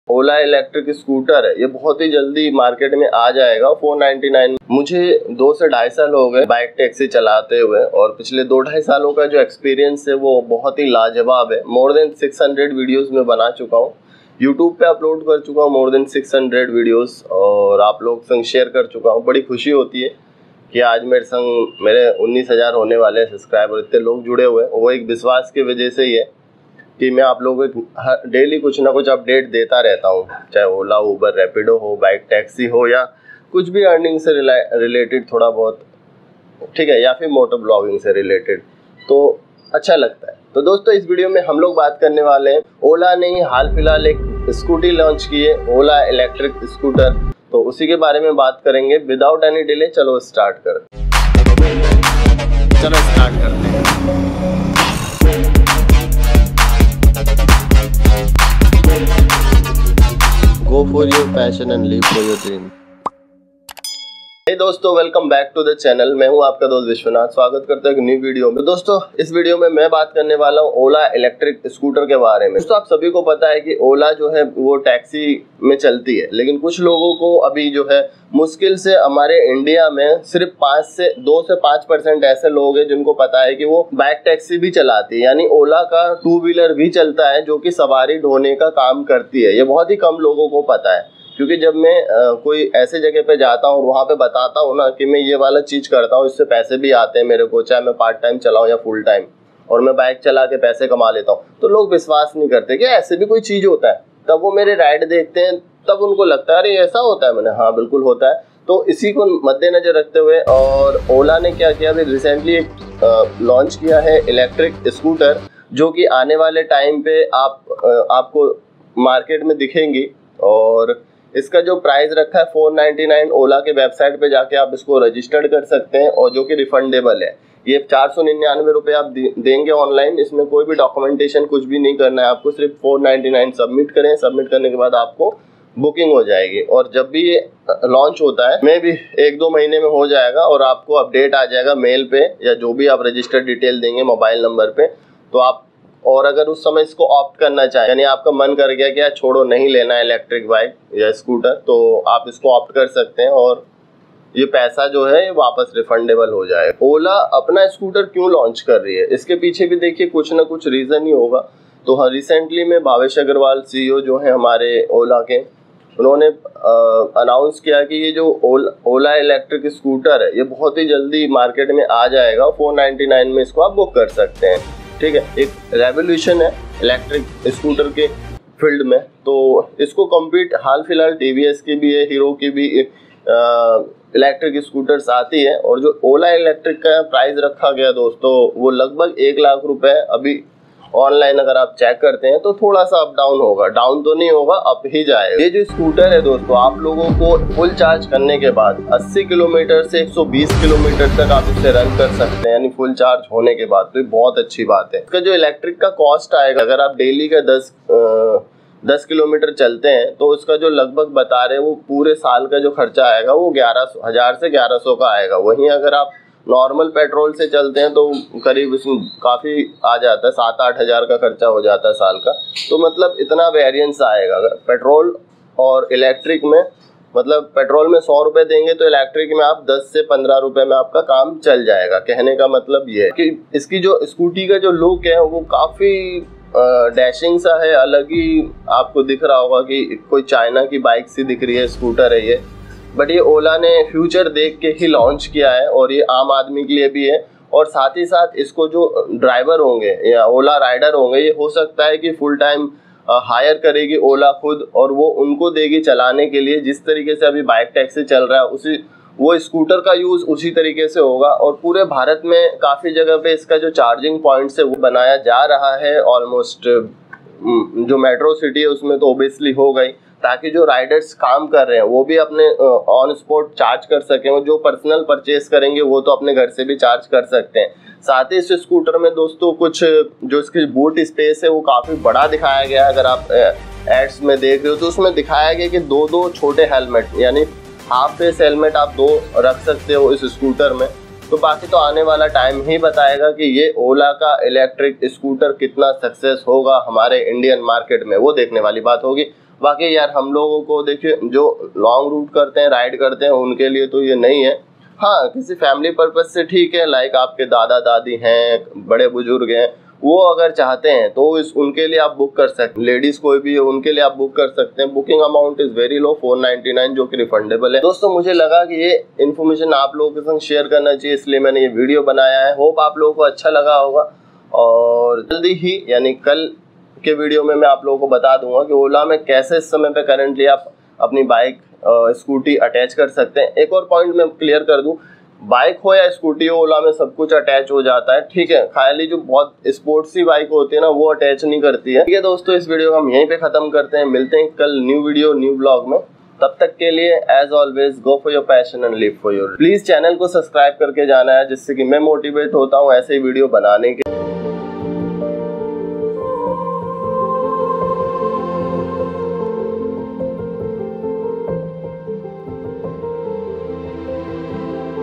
Ola Electric Scooter Launch Rs.499
Hey Dosto welcome back to another video
It's me Vishwanath Dey Your Rapido Captain

The latest tweet from Bhavish Aggarwal of Ola shows him riding an uncamouflaged Ola electric scooter that looks production ready. The tweet carried the words, “ready or not, a revolution is coming”, signifying that the launch is near. We now believe the company is working towards launching the scooter in the second half of this month